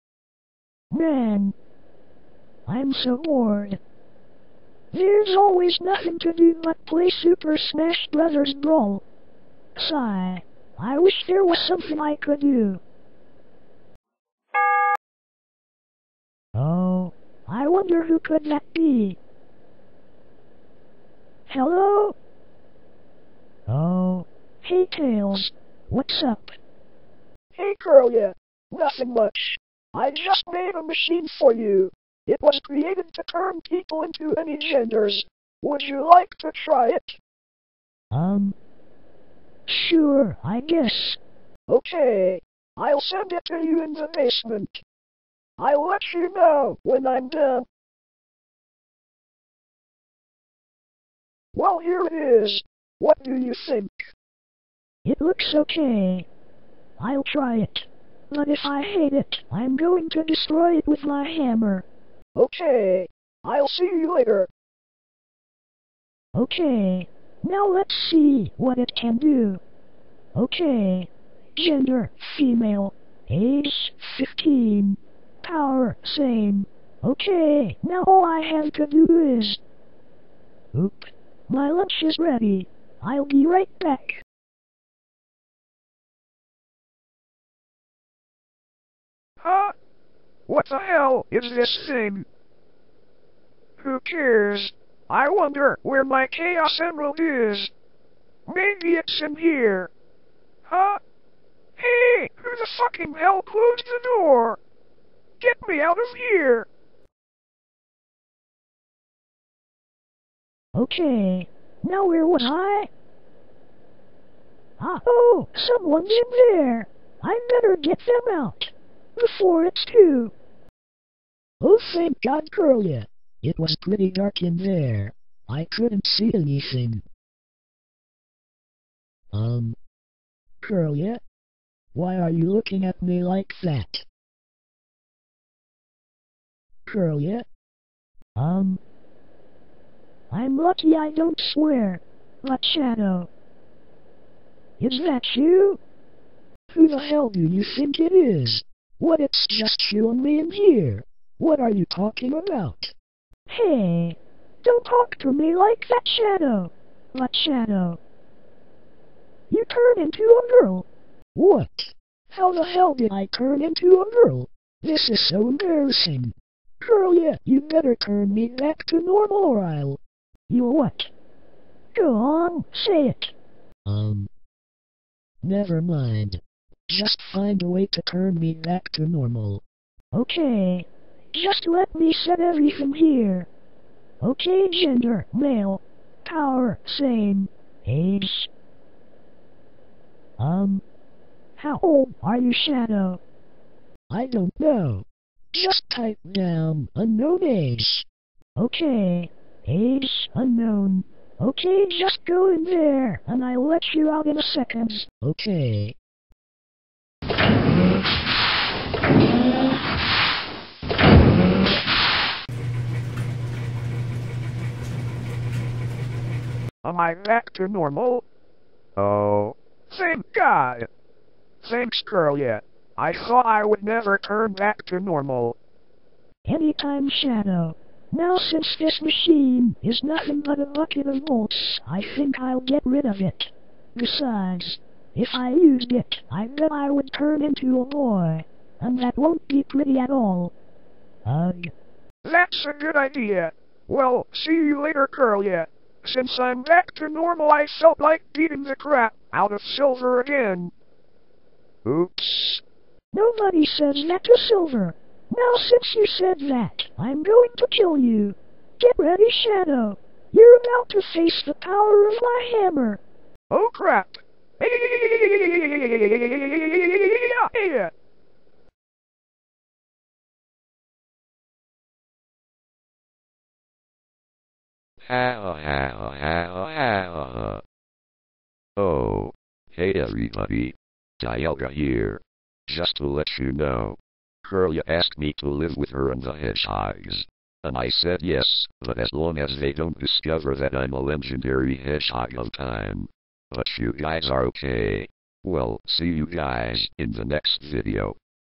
Shadow's Gender Switch
I was going to make this a 200 Subscribers video but I thought maybe I should make something a bit funnier.
well Anyways Shadow was looking for his Chaos Emerald untill he went into a machine to look for it. untill something happenO_O
( I saw a videos like this, but instead of any fc I thought of using Shadow ) Please injoy this video